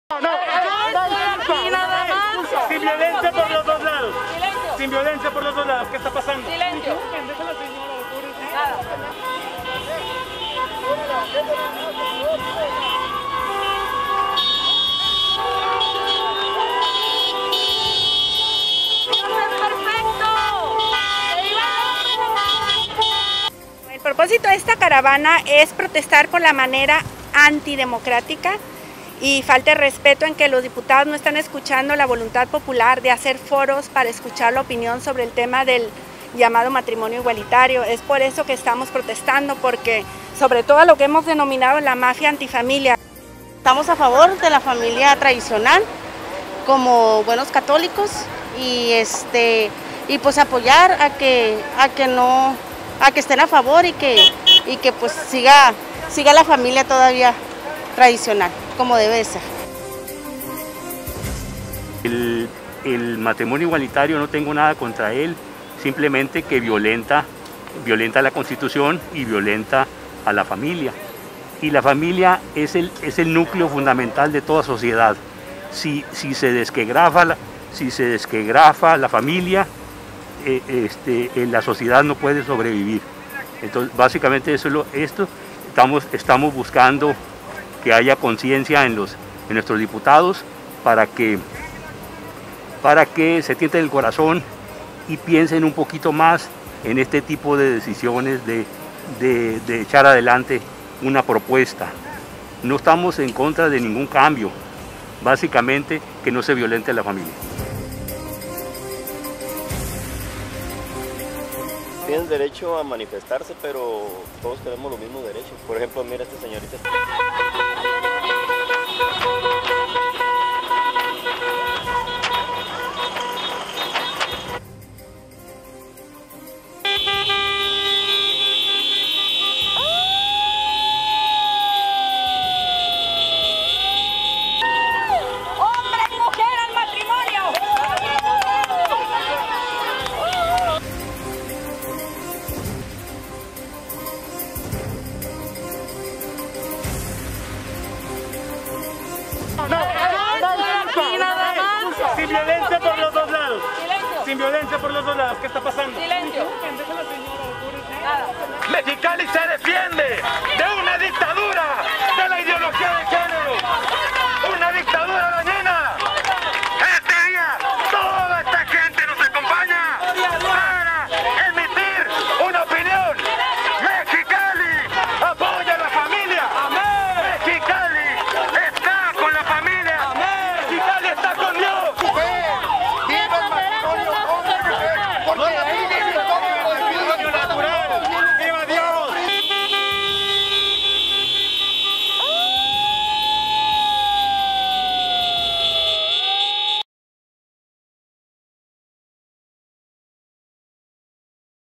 Realizan caravana a favor del matrimonio tradicional
Alrededor de 500 automovilistas acudieron a la caravana a favor del matrimonio tradicional, ayer en esta ciudad.